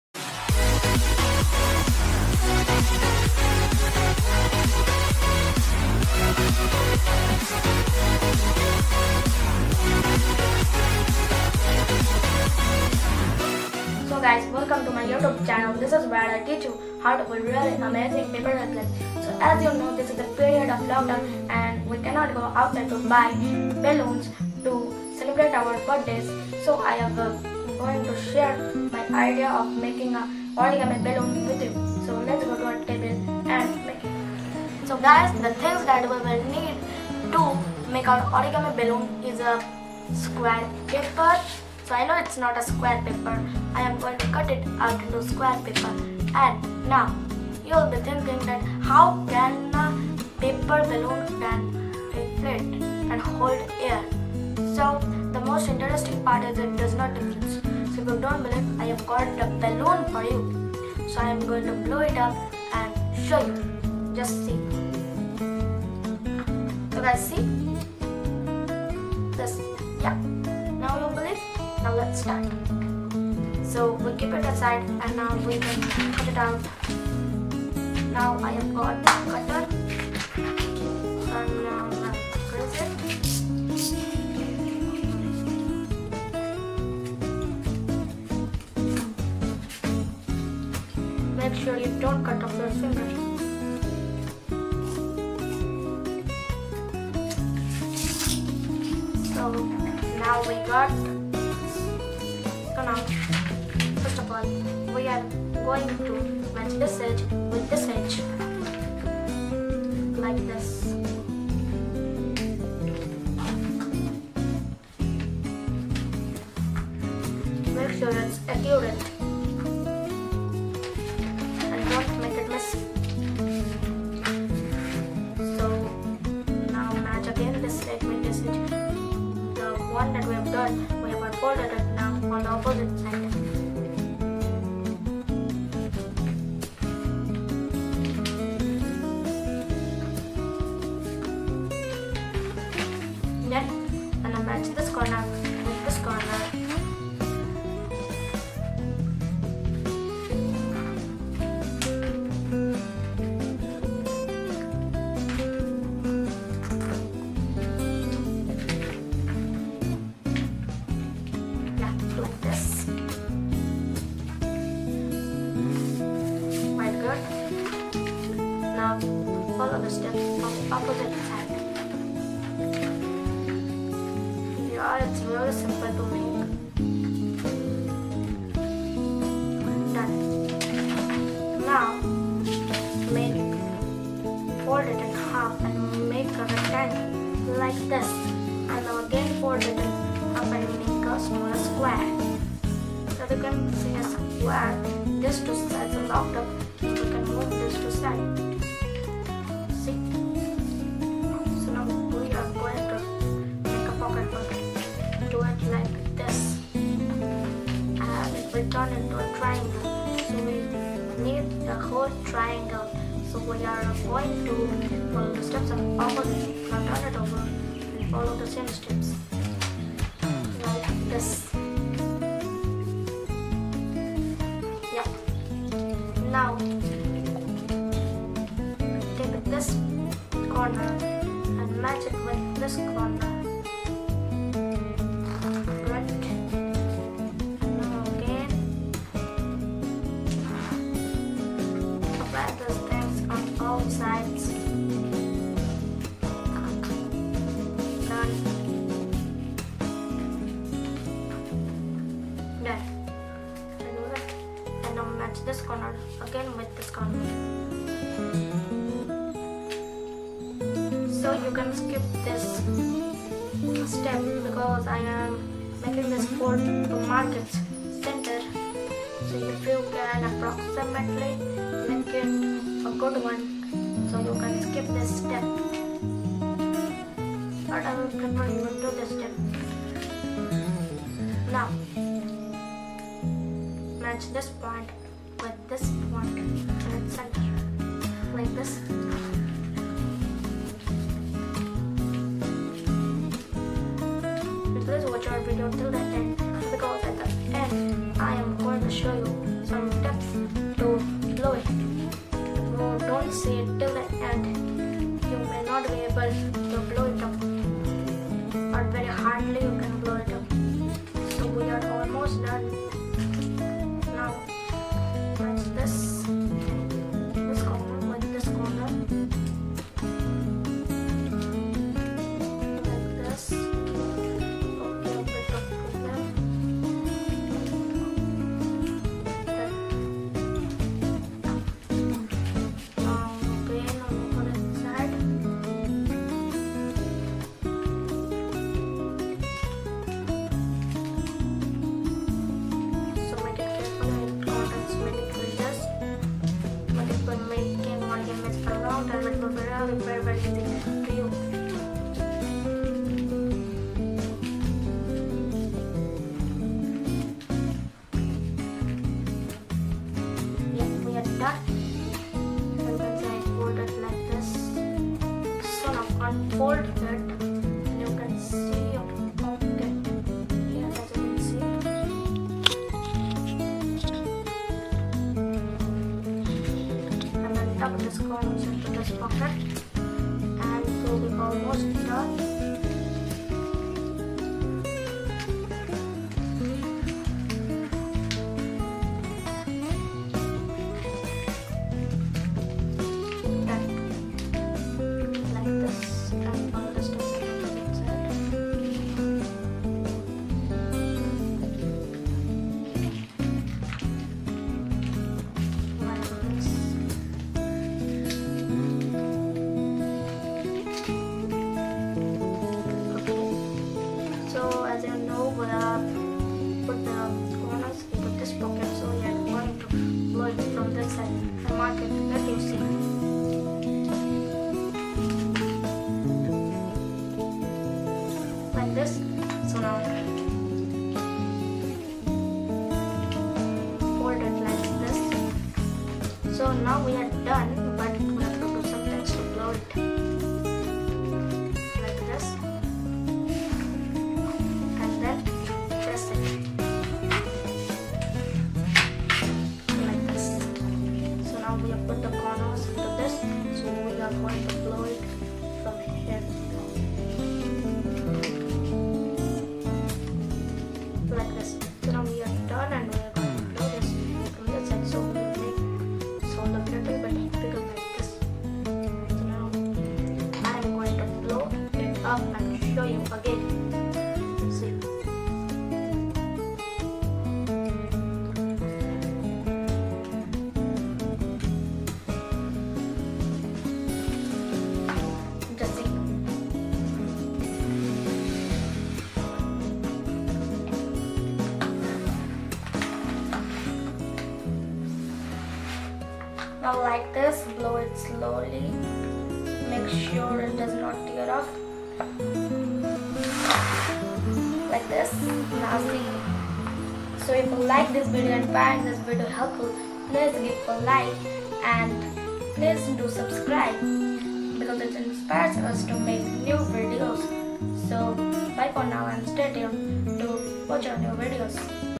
How To Fold Origami Balloon
So guys today we have learned to make a origami balloon.If guys find this video helpful please give a like and if you want to learn more origami's please subscribe to our channel.

Required Materials :
A Square Paper